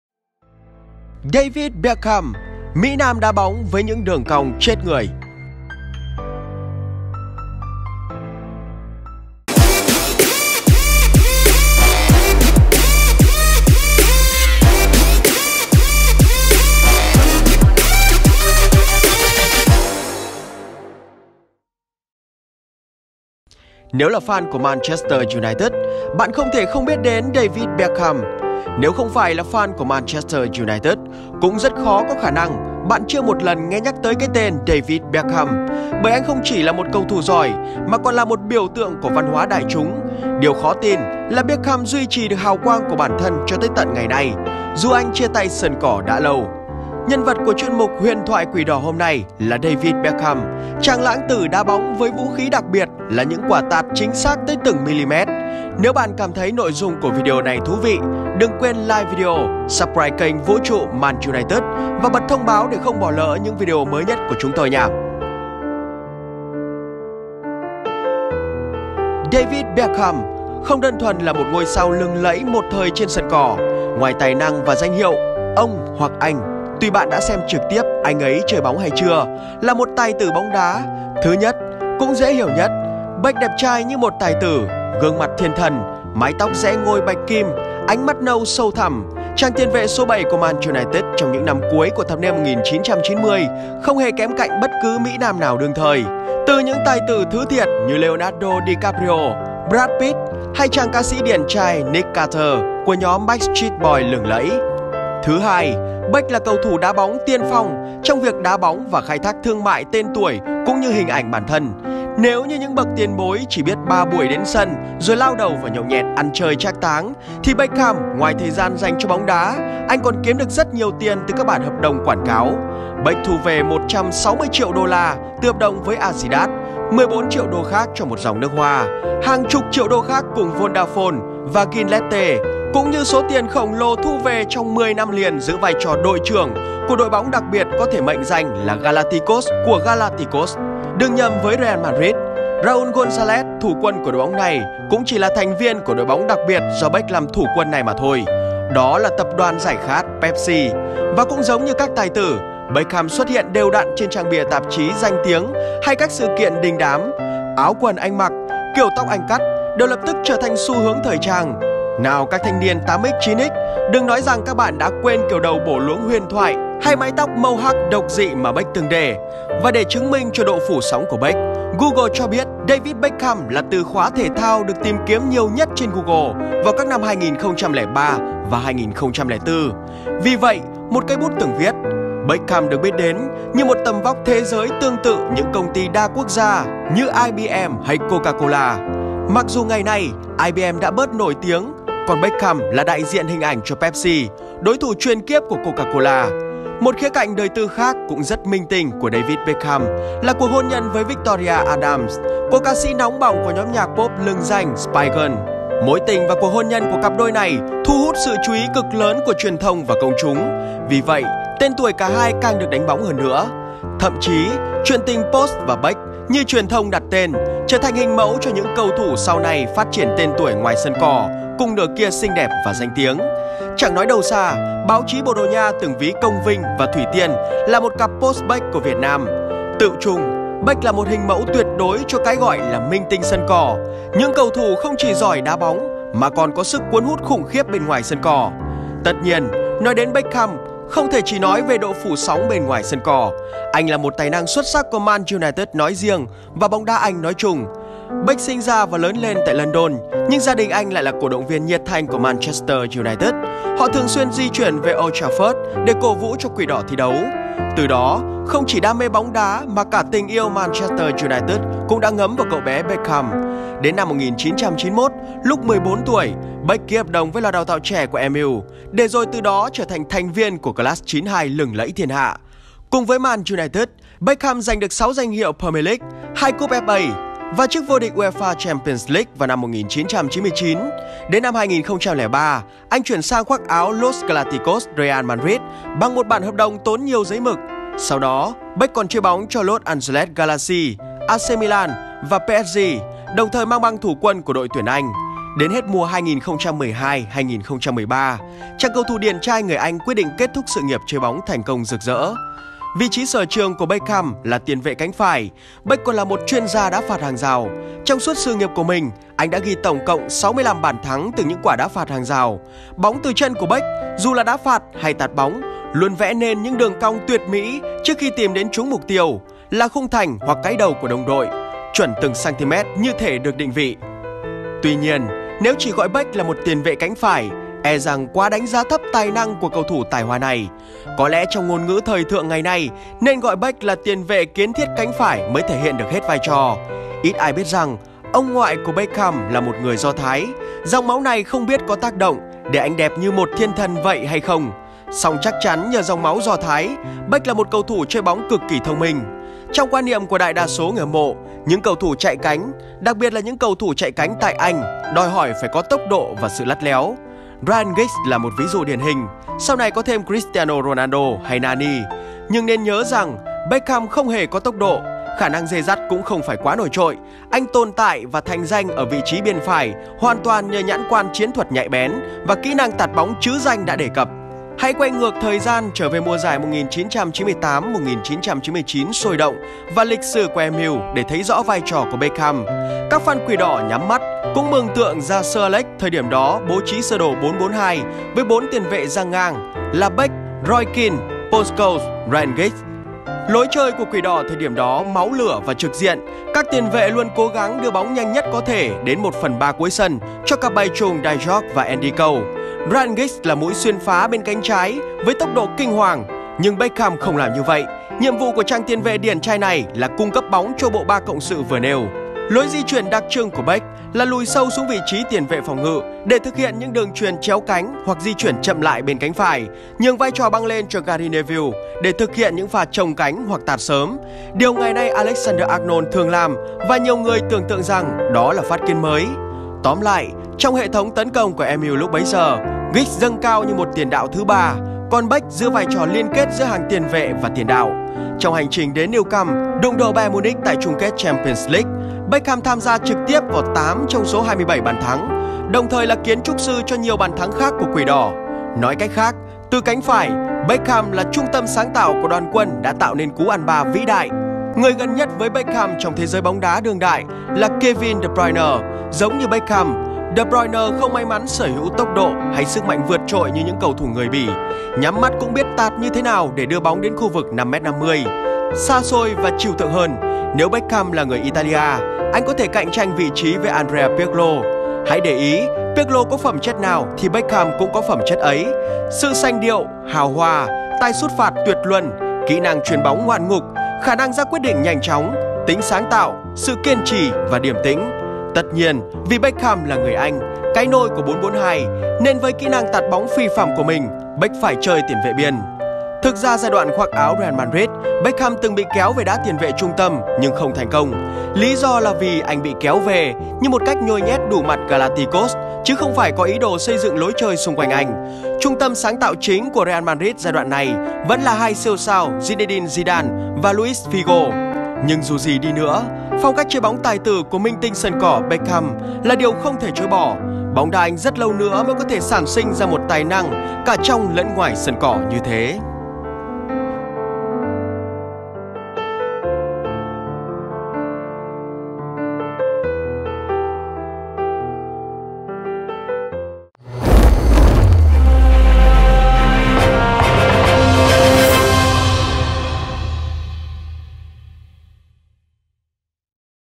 David Beckham, mĩ nam đá bóng với những đường cong chết người
Số đầu tiên trong chuyên mục "Huyền thoại Quỷ đỏ" của Vũ Trụ Man United sẽ là về David Beckham, một hiện tượng thú vị trong lịch sử bóng đá. Số 7 huyền thoại của Man United đã trở thành một biểu tượng giải trí đương đại không chỉ nhờ những quả tạt "cong như Beck", mà còn bởi vẻ đẹp trai, sức hút kỳ lạ và cuộc tình cổ tính với nàng Posh của Spice Girl.

* Subsribe kênh để không bỏ lỡ những video hấp dẫn tiếp theo nha